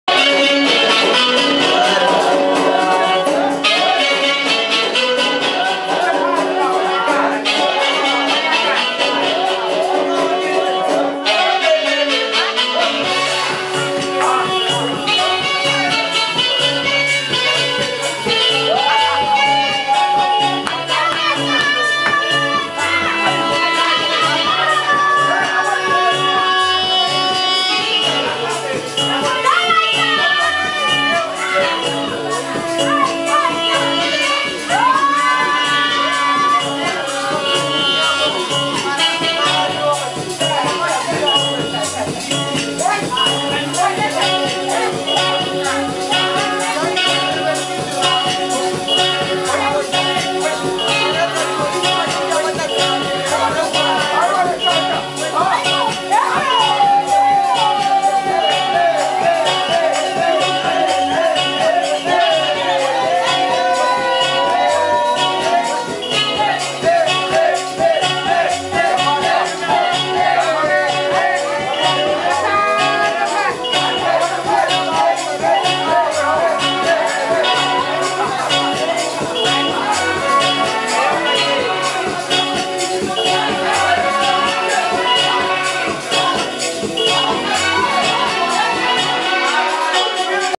U.I.E rocking UJ students @ Club 10 Barnes Part #2
What Engineering students are capable of, CAMPUS SHUTDOWN 16 June 2013